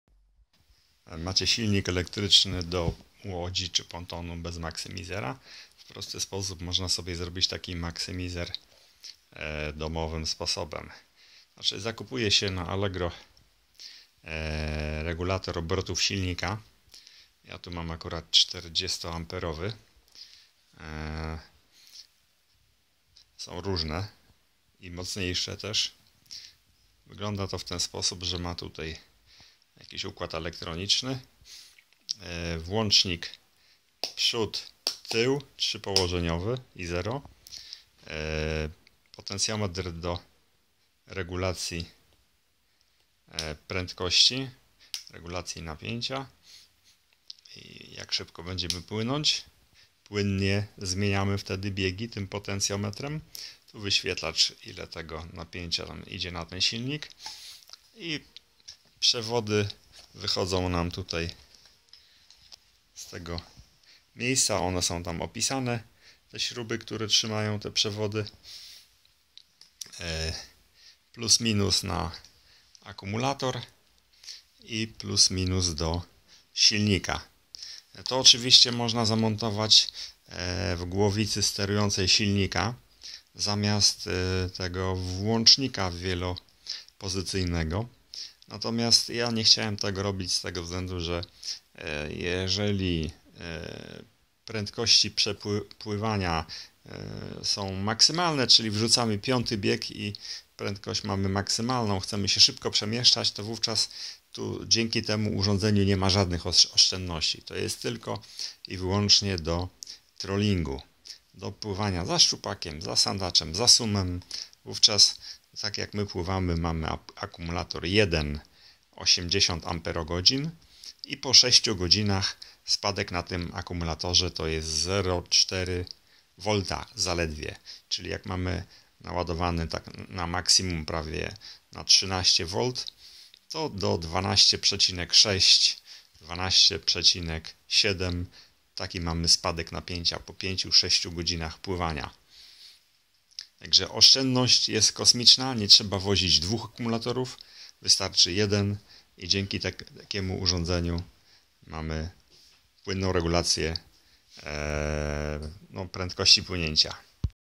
Maksymizer do silnika elektrycznego.
Jeśli macie silnik elektryczny do łodzi czy pontonu bez maksymizera, to zamiast kupować drugi i drogi silnik, kupcie sobie pudełko plastikowe i regulator obrotów silnika PWM w cenie ok 20 zł na allegro. Koszt inwestycji to około 40 zł z kablami i zaciskami. Nie potrzeba wozić dwóch akumulatorów bo oszczędności prądu się kolosalne. Żeby rozładować akumulator 80 Ah do poziomu napięcia 12V  przy dwóch osobach i pontonie 320 cm, trzeba by  trolingować non stop 10 -12 godzin.